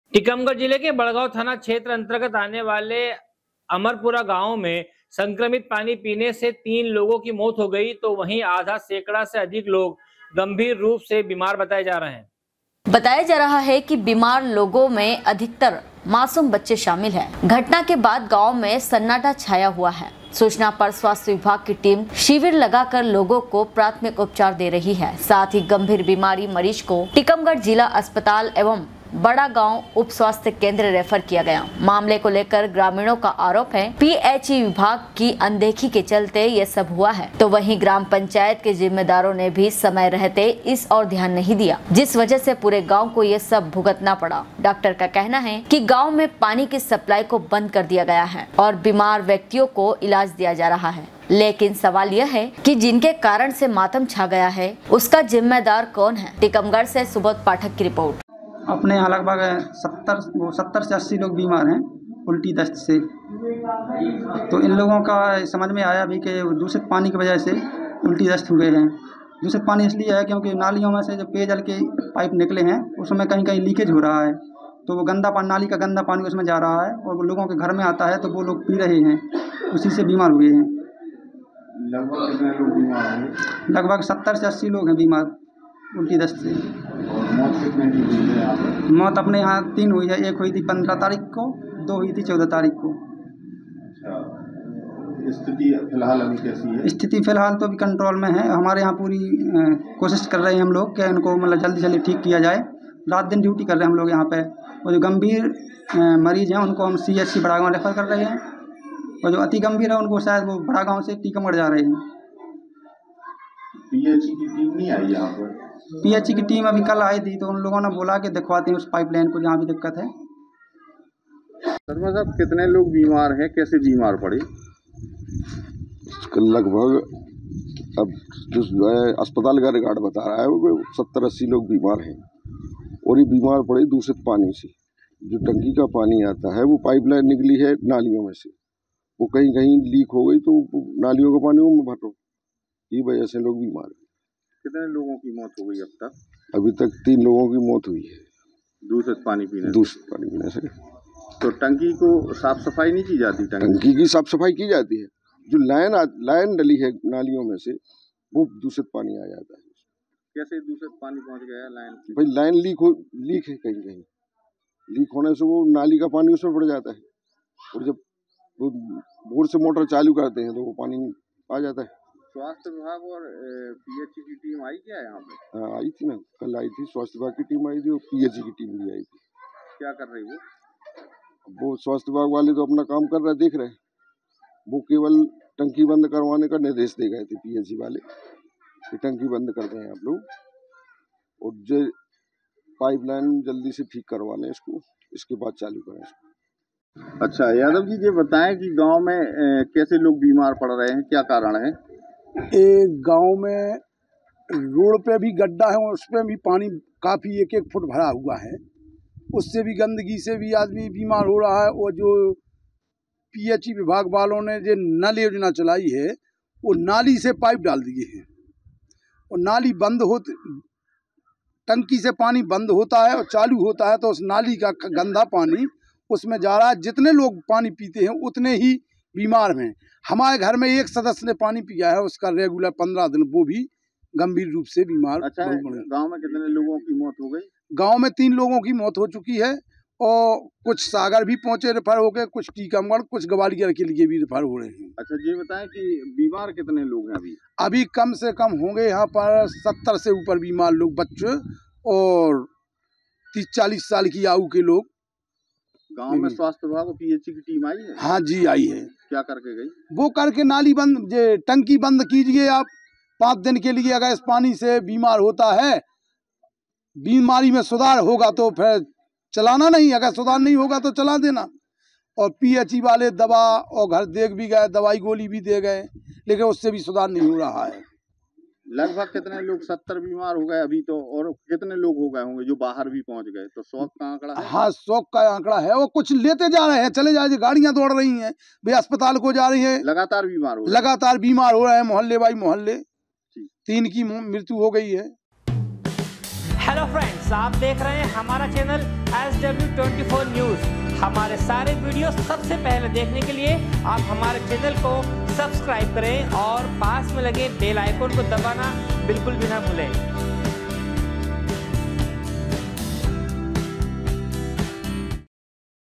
बड़ागांव थाना क्षेत्र अंतर्गत आने वाले अमरपुर गांव में संक्रमित पानी पीने से तीन लोगों की मौत हो गई
टिकमगढ जिले के बड़ागांव थाना क्षेत्र अंतर्गत आने वाले अमरपुर गांव में संक्रमित पानी पीने से तीन लोगों की मौत हो गई है तो वही आधा सैकड़ा से अधिक लोग गंभीर रूप से बीमार बताए जा रहे हैं
टीकमगढ़ सुबोध पाठक की रिपोर्ट।